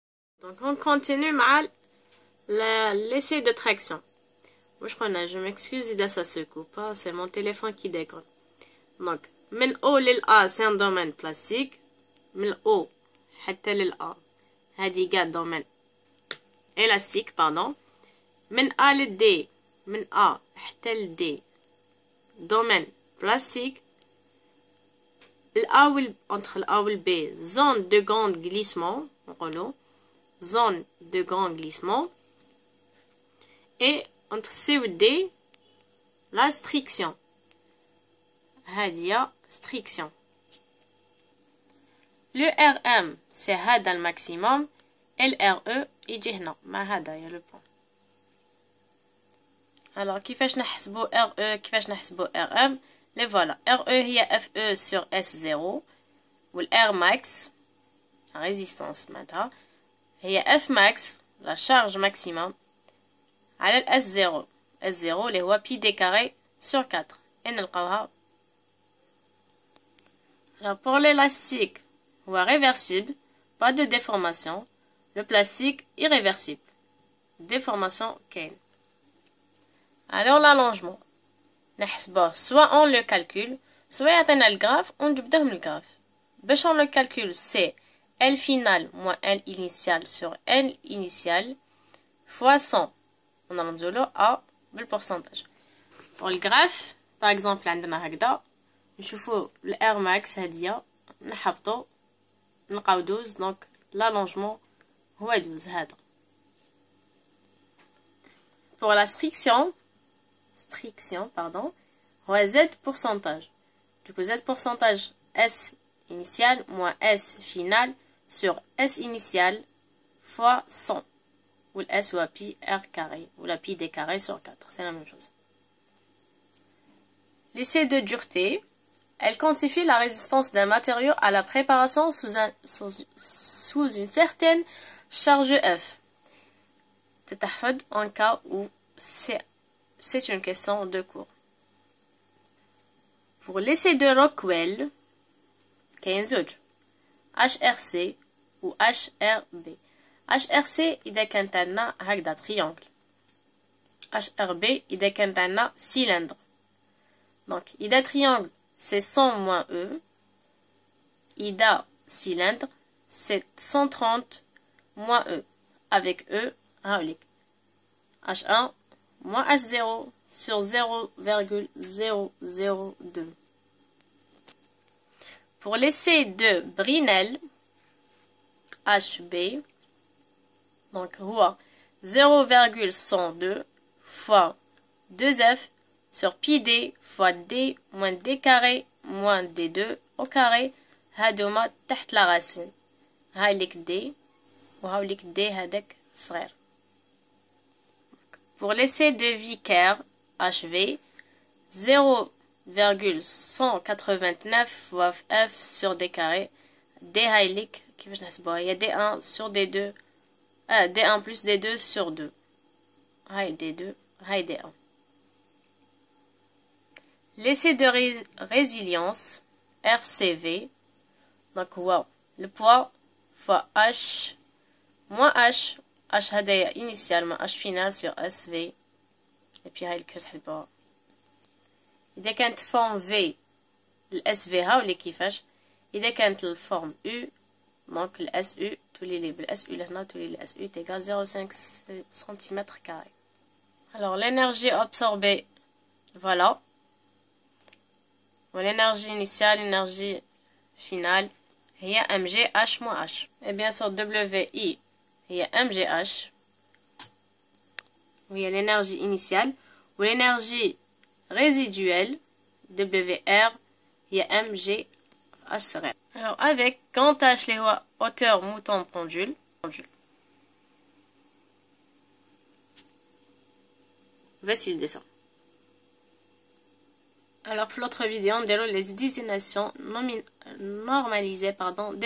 TECHNIQUE DE BASE / LES ESSAIS DE : DURETÉ, ROCKWELL, BRINELL, VICKERS, DE RÉSILIENCE ( PARTIE 3 )
PS : y a plus de vidéos sur ma chaîne ne les ratez pas !